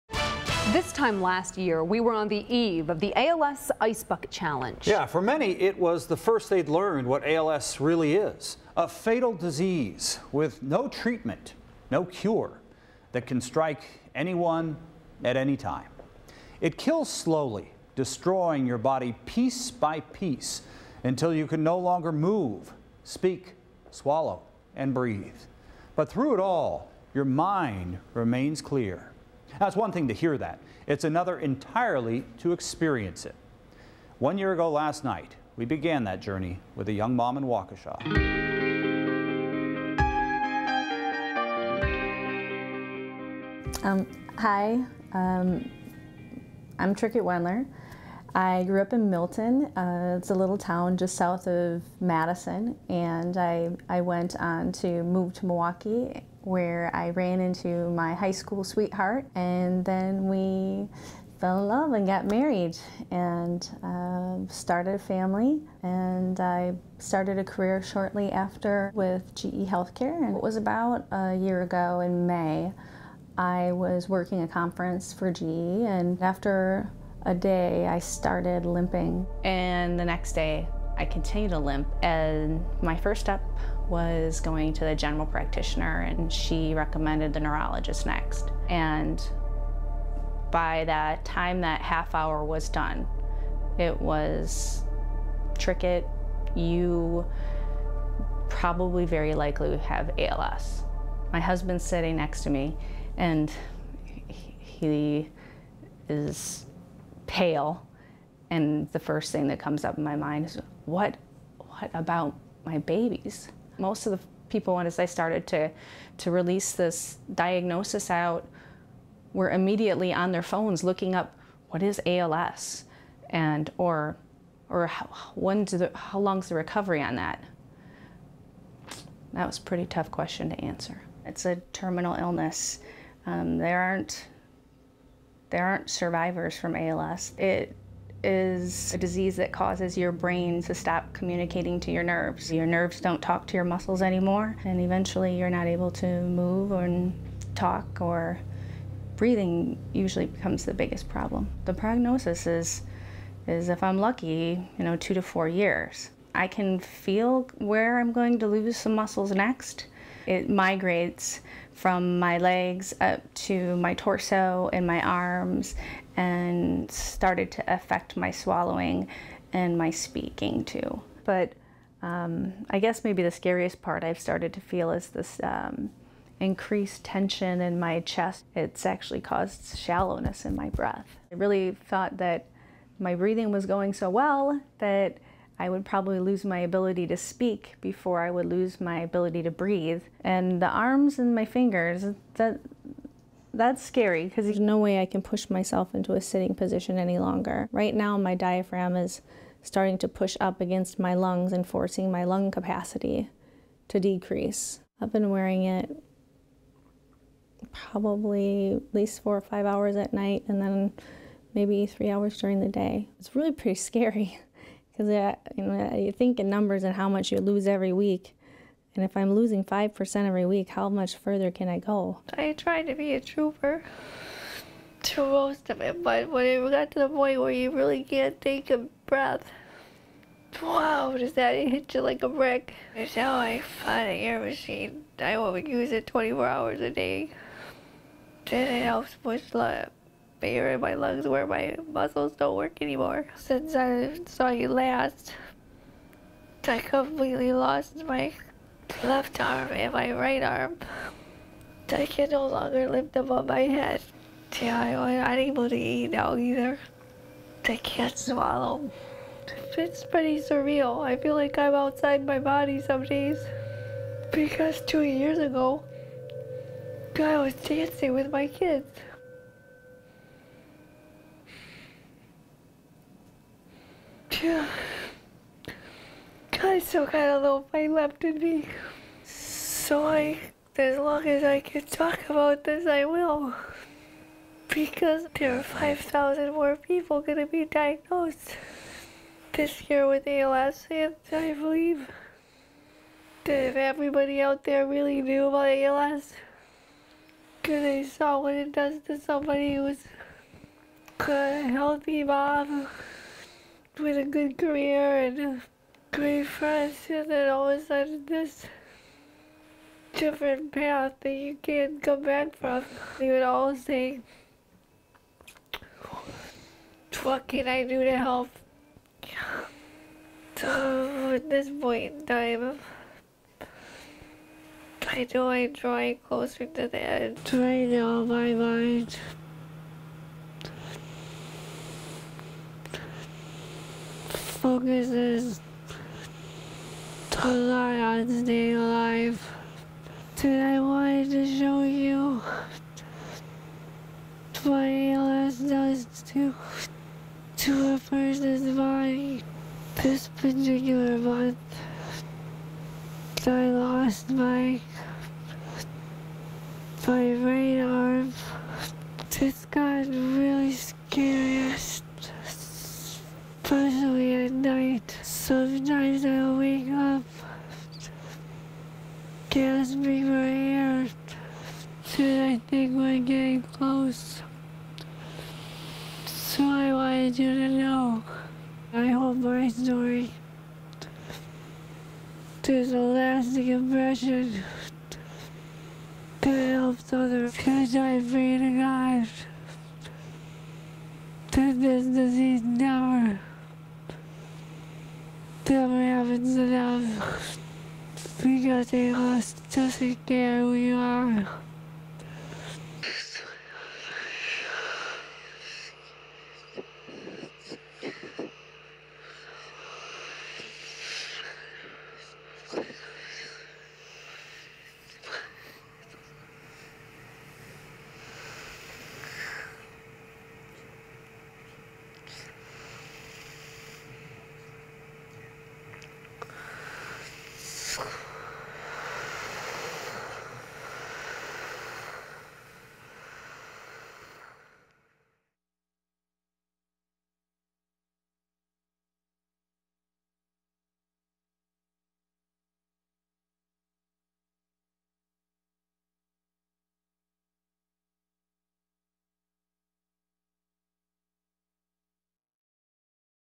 Trickett's Story
Many people became aware of ALS through the Ice Bucket Challenge.  Few, however, still really understand the disease and what it does.  This intimate story reveals one woman's moving and unforgettable journey with ALS.